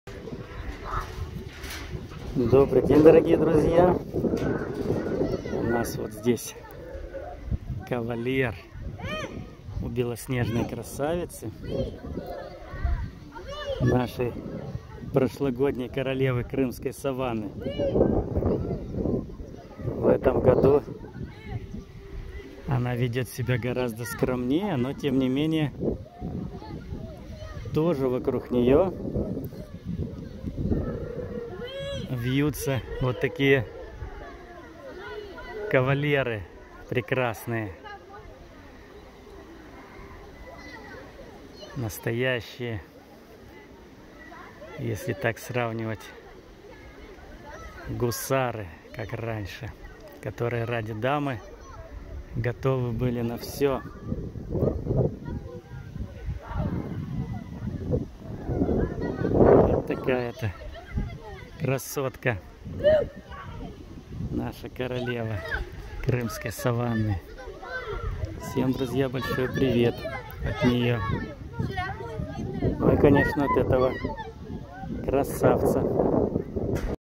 Белая красавица со своим телохранителем) Тайган
Белая красавица львица и ее верный телохранитель на львином пляже.